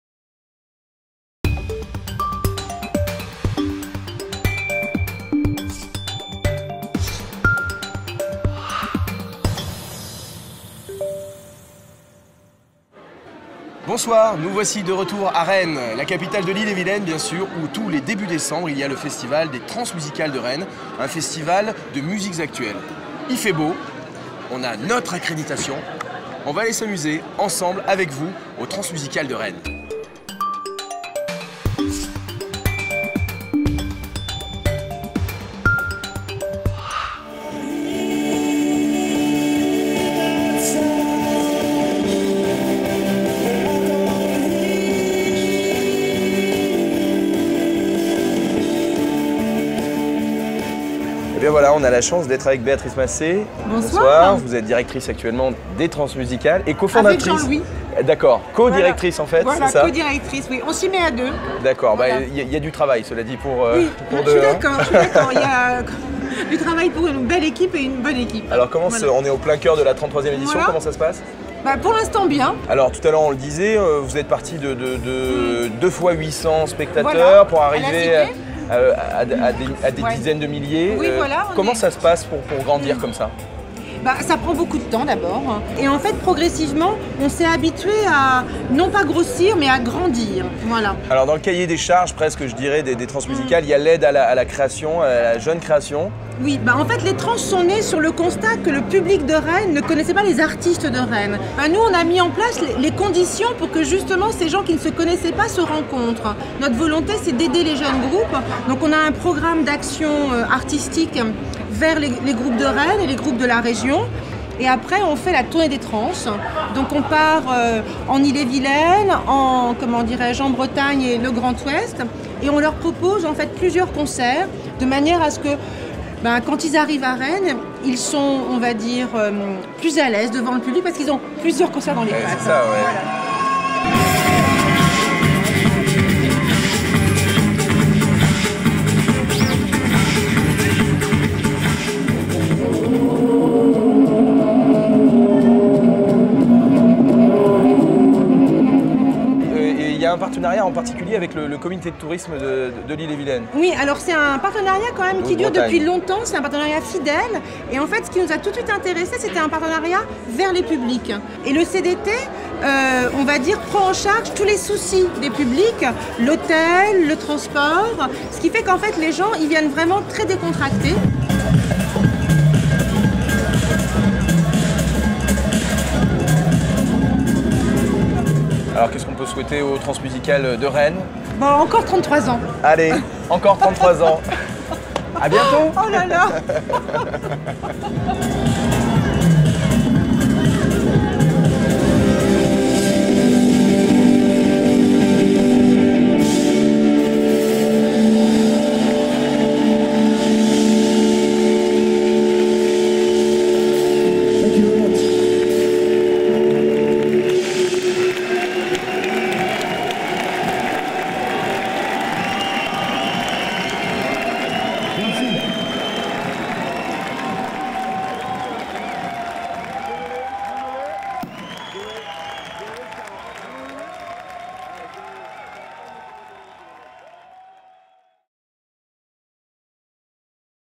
Les Transmusicales au Parc Expo
Au Parc Expo où les concerts continuent jusqu'au bout de la nuit, Philippe rencontrera  la co-directrice des Transmusicales, Béatrice Macé, et vous montrera l'éclectisme de la sélection musicale des Transmusicales avec des artistes du monde entier : le rock de Robin Foster, le reggae-soul d'Hollie Cook, les DJ NekoChan et Todd Terje ainsi que le saxophoniste Colin Stetson du groupe Arcade Fire. Une invitation à découvrir les Tranmusicales 2012 qui se dérouleront le 1er week-end de décembre.